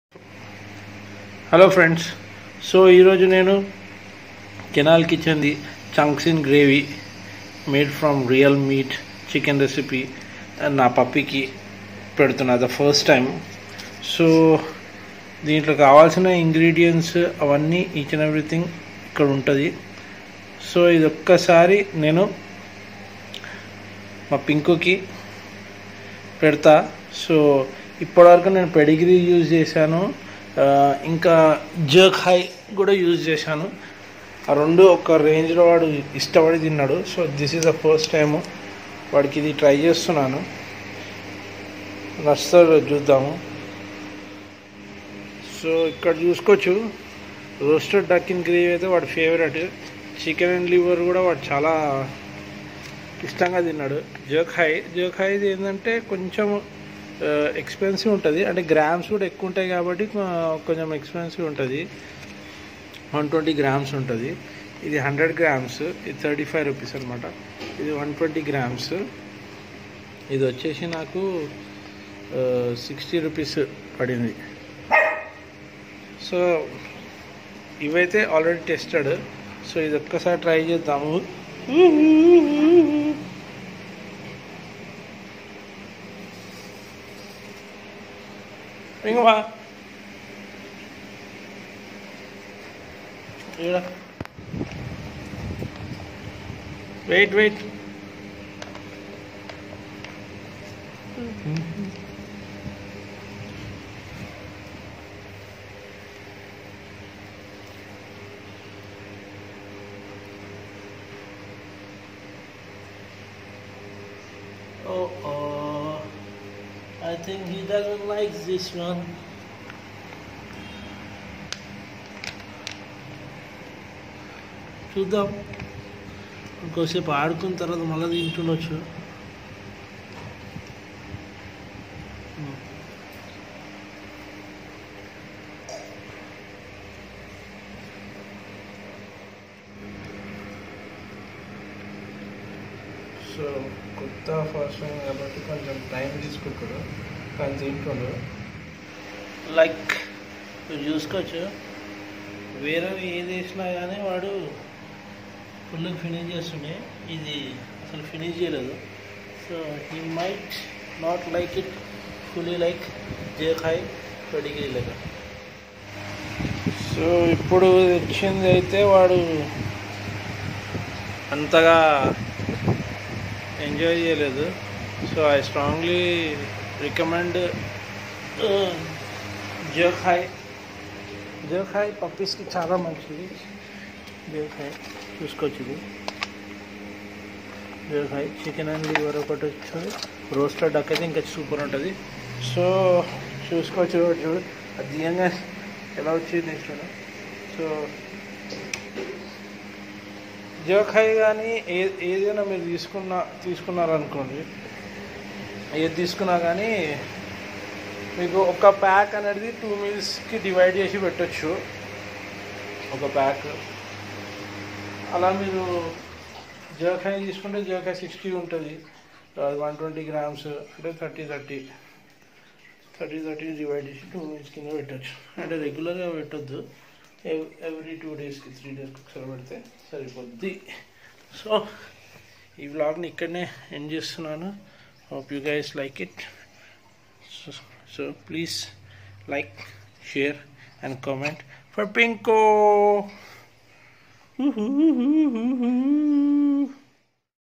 Kennel Kitchen Dog Food Genuine Review
Features
Key Features

Suitable for all ages, breeds and sizes
Puppy and Adult Recipe made with Regionally sourced premium quality deboned chicken and chicken liver
Preservative Free
Free from Artificial Flavours and colours
High in natural Omega 3 and 6 Fatty Acids

Ingredients
Ingredients